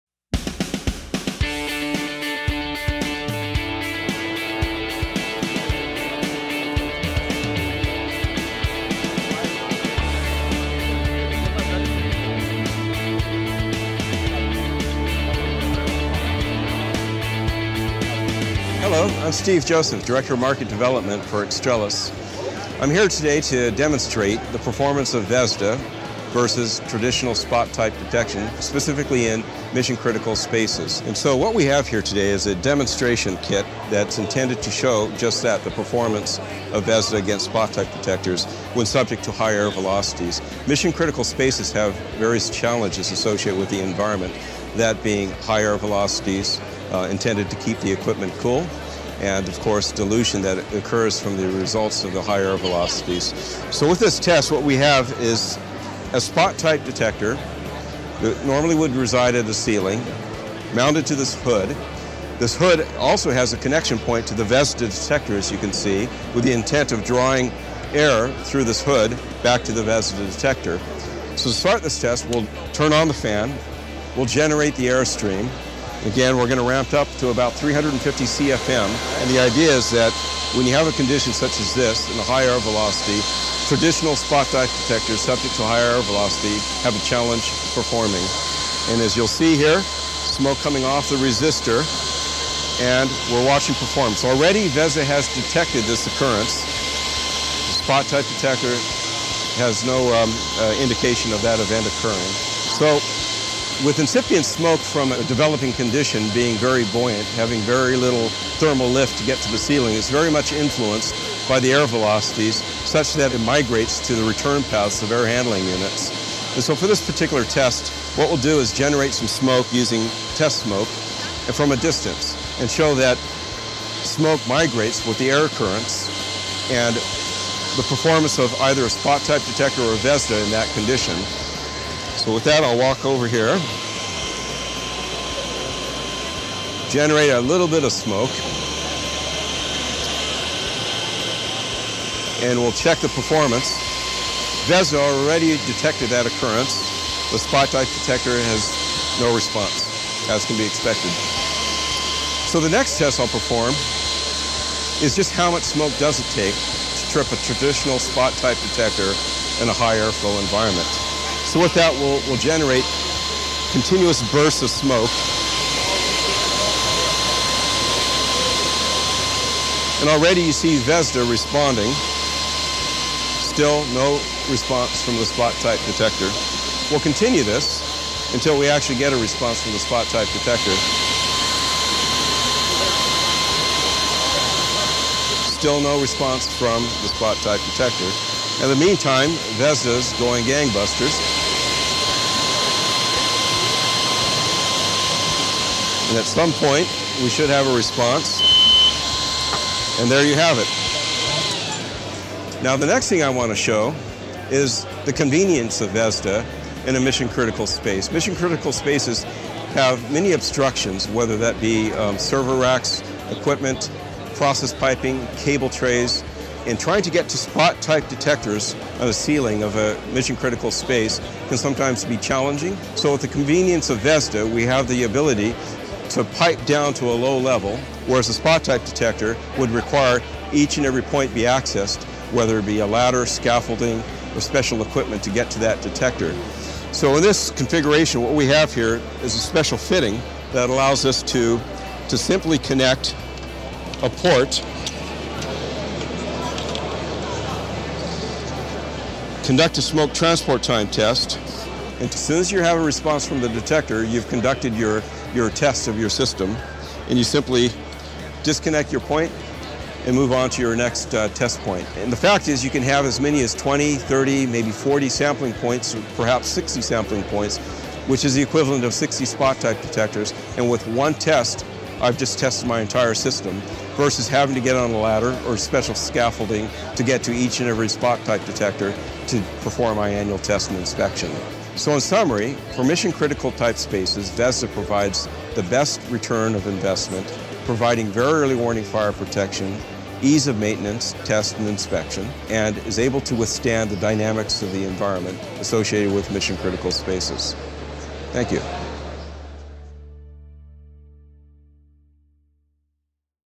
VESDA vs Spot Smoke Detection in High Airflow Environments
This video clearly demonstrates the superior and very early warning capabilities of VESDA versus spot smoke detectors in a high airflow environment (like data centers, switching centers, etc).